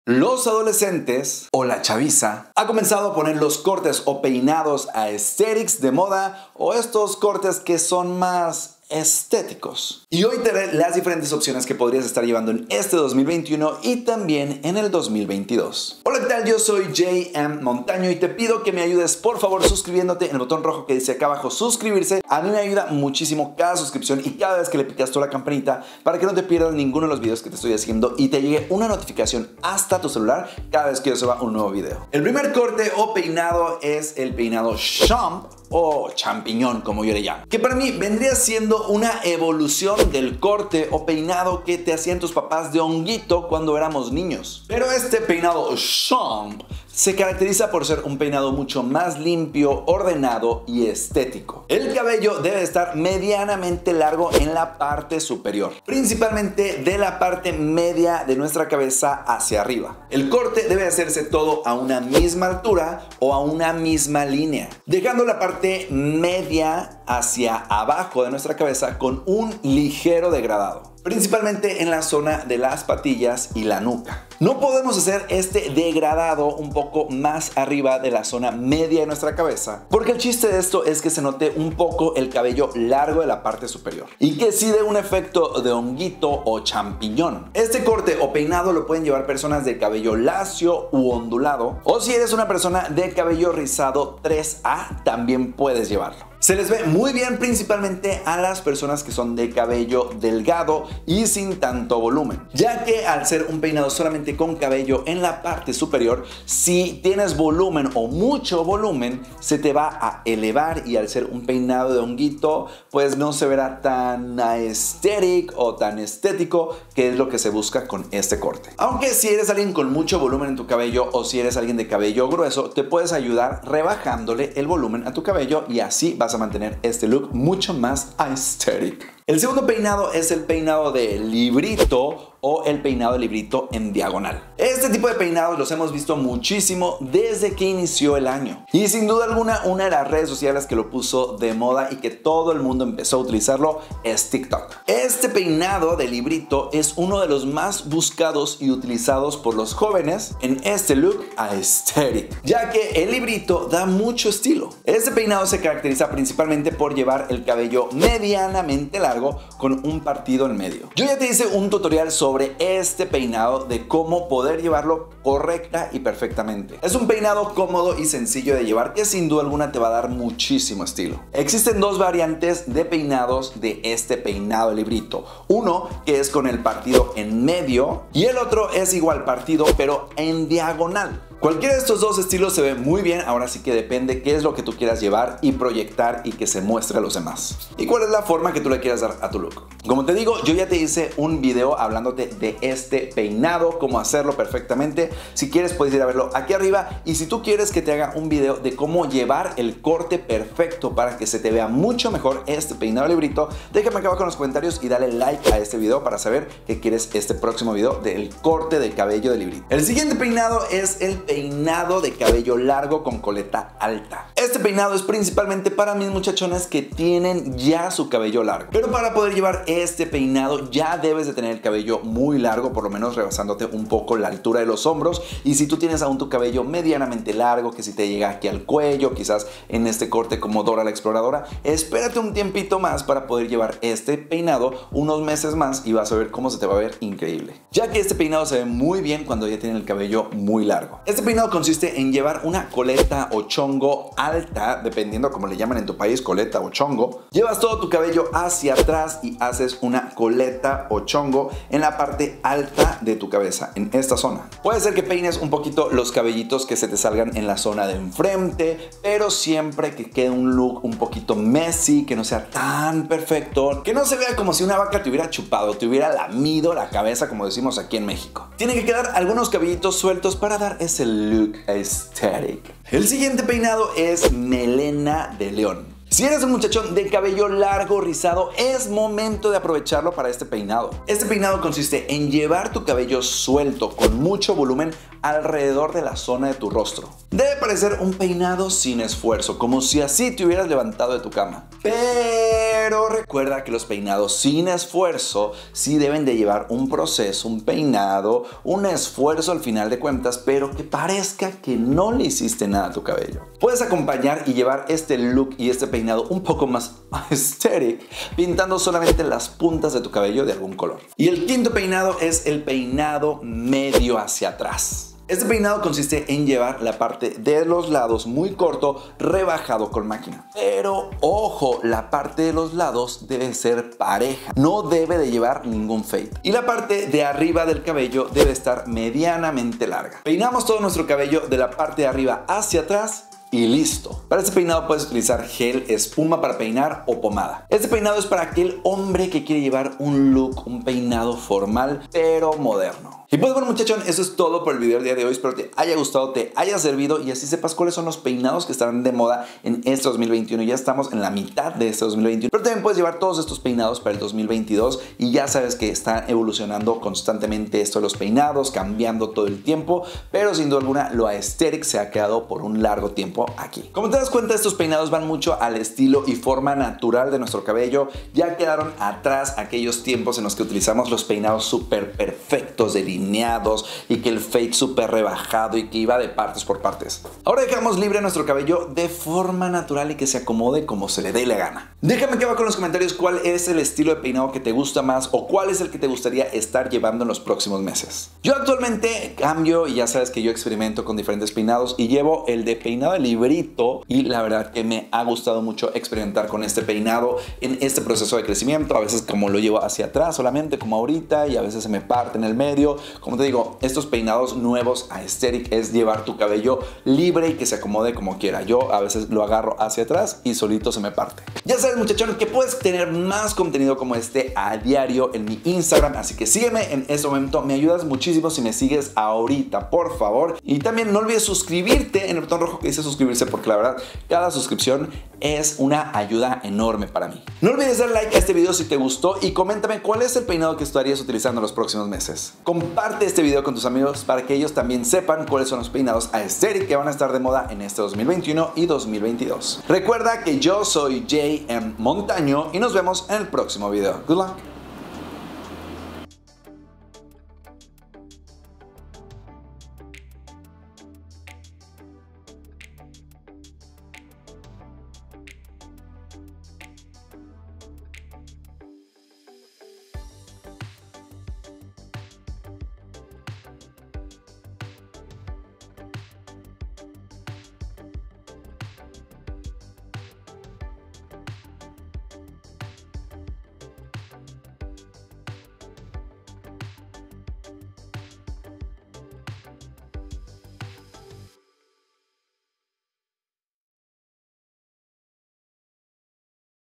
PEINADOS AESTHETIC PARA ADOLESCENTES 2021/2022 - J.M. Montaño
Los peinados AESTHETIC están de moda en este 2021 y estarán hasta el 2022 o incluso llegaron para quedarse. Por eso te comparto los mejores peinados para adolescentes y jóvenes  que puedes llevar para diferentes tipos de cabellos y estilos.